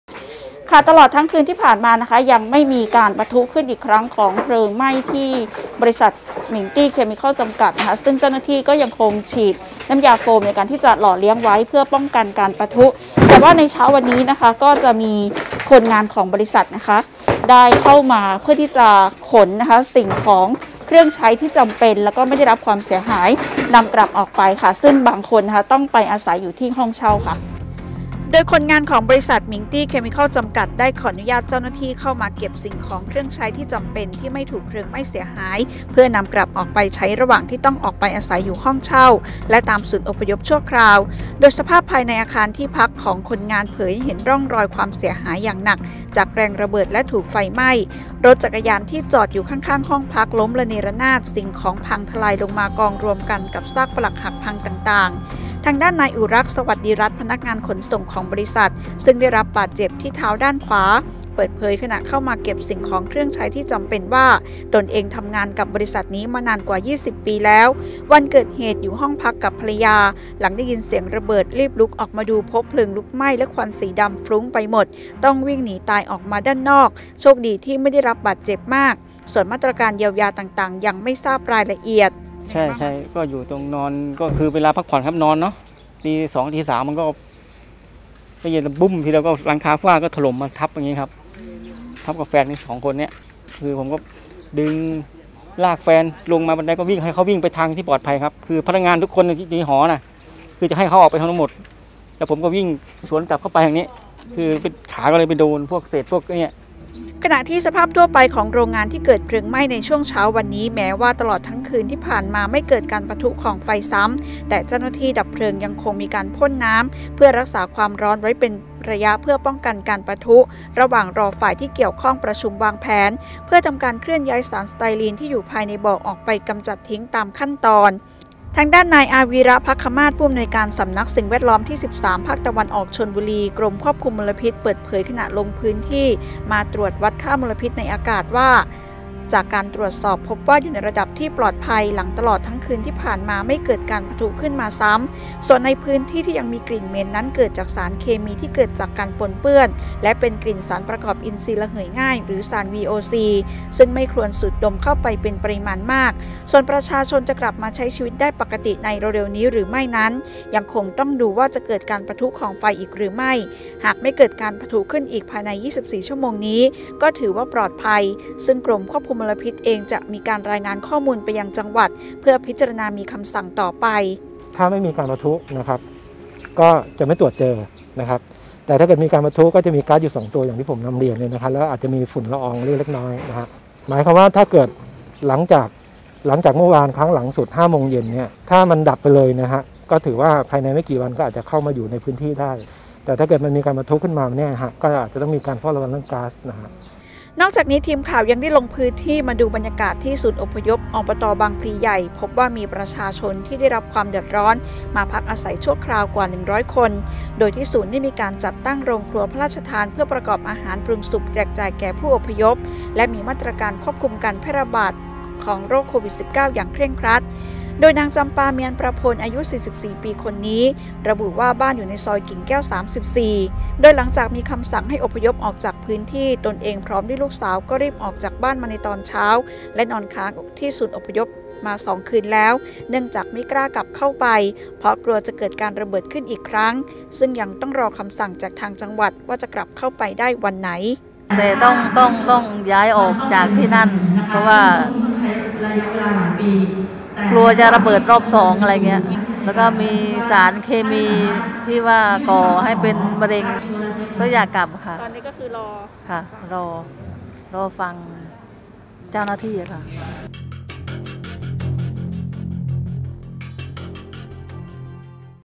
ชาวบางพลีลุ้นกลับเข้าบ้าน หลังไฟไม่ปะทุซ้ำ
โดยคนงานของบริษัท หมิงตี้ เคมิคอล จำกัด ได้ขออนุญาตเจ้าหน้าที่เข้ามาเก็บสิ่งของเครื่องใช้ที่จำเป็นที่ไม่ถูกไฟไหม้เสียหายเพื่อนำกลับออกไปใช้ระหว่างที่ต้องออกไปอาศัยอยู่ห้องเช่าและตามศูนย์อพยพชั่วคราว โดยสภาพภายในอาคารที่พักของคนงานเผยให้เห็นร่องรอยความเสียหายอย่างหนักจากแรงระเบิดและถูก ไฟไหม้ รถจักรยานที่จอดอยู่ข้างๆห้องพักล้มระเนระนาด สิ่งของพังทลายลงมากองรวมกับซากปรักหักพังต่างๆ

ด้าน นายอุรักษ์ สวัสดิ์รัตน์ พนักงานขนส่งของ บริษัทซึ่งได้รับบาดเจ็บที่เท้าด้านขวาเปิดเผย ขณะเข้ามาเก็บสิ่งของเครื่องใช้ที่จำเป็นว่าตนเองทำงานกับบริษัทนี้มานานกว่า 20 ปีแล้ว วันเกิดเหตุอยู่ห้องพักกับภรรยาหลังได้ยินเสียงระเบิดรีบลุกออกมาดูพบเพลิงลุกไหม้และควันสีดำไปหมด ต้องวิ่งหนีตายออกมาด้านนอกโชคดีที่ไม่บาดเจ็บมากนัก ส่วนมาตรการเยียวยาต่างๆยังไม่ทราบรายละเอียด

"ก็คือเวลาพักผ่อนเวลานอนเนอะตี 2 ตี 3 ก็ได้ยินเสียงบึ้ม หลังคาฝ้าเพดานก็ถล่มมาทับผมกับแฟนสองคน คือผมก็ดึงลากแฟนลงมาบันไดให้เขาวิ่งไปทางที่ปลอดภัย พนักงานทุกคนในห้องทุกคน คือจะให้เขาออกไปทั้งหมด แล้วผมก็วิ่งสวนกลับเข้าไปทางนี้ คือขาก็เลยไปโดนพวกเศษต่างๆ บาดเจ็บ"

ขณะที่ สภาพทั่วไปของโรงงานที่เกิดเพลิงไหม้ในช่วงเช้าวันนี้ แม้ว่าตลอดทั้งคืนที่ผ่านมาไม่เกิดการปะทุของไฟซ้ำ แต่เจ้าหน้าที่ดับเพลิงยังคงมีการฉีดพ่นน้ำ เพื่อรักษาความร้อนไว้เป็นระยะป้องกันการปะทุ ระหว่างรอฝ่ายที่เกี่ยวข้องประชุมวางแผนเพื่อทำการเคลื่อนย้ายสารสไตรีนที่อยู่ภายในบ่อออกไปกำจัดทิ้งตามขั้นตอน

ด้าน นายอาวีระ ภัคมาตร์ ผู้อำนวยการสำนักสิ่งแวดล้อมที่ 13 ภาคตะวันออก (ชลบุรี) กรมควบคุมมลพิษ เปิดเผยขณะลงพื้นที่ตรวจวัดสารพิษทางอากาศ พบว่าค่ามลพิษในอากาศอยู่ระดับที่ปลอดภัยหลังตลอดทั้งคืนที่ผ่านมาไม่เกิดการปะทุขึ้นอีกส่วนในพื้นที่ยังมีกลิ่นเหม็นเกิดจากสารเคมีที่เกิดการปนเปื้อนและเป็นกลิ่นสารประกอบอินทรีย์ระเหยง่าย หรือ สาร VOC ซึ่งไม่ควรสูดในปริมาณมาก ส่วนประชาชนจะกลับมาใช้ชีวิตปกติได้ในเร็วๆนี้หรือไม่นั้น ต้องดูว่าจะเกิดการปะทุของไฟอีกหรือไม่ หากไม่เกิดการปะทะอีกภายใน 24 ชม. ก็ถือว่าปลอดภัย ซึ่งกรมมลพิษจะรายงานข้อมูลไปยังจังหวัดเพื่อพิจารณามีคำสั่งต่อไป

"ถ้าไม่มีการปะทุนะครับ ก็จะไม่ตรวจเจอแต่ถ้ามีปะทุ ก็จะเจอสาร 2 ตัวอย่างที่ผมนำเรียนและไป อาจจะมีฝุ่นละอองเล็กน้อยหมายความว่าถ้าเกิดหลังจากเมื่อวานครั้งล่าสุดตอน 5 โมงเย็น ถ้ามันดับไปเลยนะครับภายในไม่กี่วันก็อาจจะเข้ามาอยู่ในพื้นที่ได้แต่ถ้าเกิดมีการปะทุขึ้นมาก็จะต้องมีการเฝ้าระวังเรื่องก๊าซ"

นอกจากนี้ ทีมข่าวได้ลงพื้นที่ดูบรรยากาศที่ศูนย์อพยพ อบต.บางพลีใหญ่ มีประชาชนได้รับความเดือดร้อนมาพักอาศัยชั่วคราวกว่า 1 ร้อยคน โดยที่ศูนย์ได้มีการจัดตั้งโรงครัวพระราชทาน เพื่อประกอบอาหารปรุงสุกแจกจ่ายแก่ผู้อพยพ และมีมาตรการการควบคุมการแพร่ระบาดโรคโควิด-19 อย่างเคร่งครัด

โดย นางจำปา เมียดประโพน อายุ 44 ปี ชาวบ้าน ซอยกิ่งแก้ว 34 เปิดเผยว่า หลังมีคำสั่งให้อพยพจากทางจังหวัด ตนเองพร้อมลูกสาวก็รีบออกจากบ้านมาตอนเช้าและนอนค้างคืนที่ศูนย์อพยพมา 2 คืนแล้ว เนื่องจากไม่กล้ากลับเข้าไปเพราะกลัวจะเกิดการระเบิดขึ้นอีกครั้ง ต้องรอคำสั่งจากจังหวัด

"เลยต้องย้ายออกจากที่นั่นเพราะว่ากลัวจะระเบิดรอบสอง และก็มีสารเคมีที่ว่าก่อให้เป็นมะเร็ง ก็อยากกลับเข้าไปในบ้านตอนนี้ก็รอฟังเจ้าหน้าที่"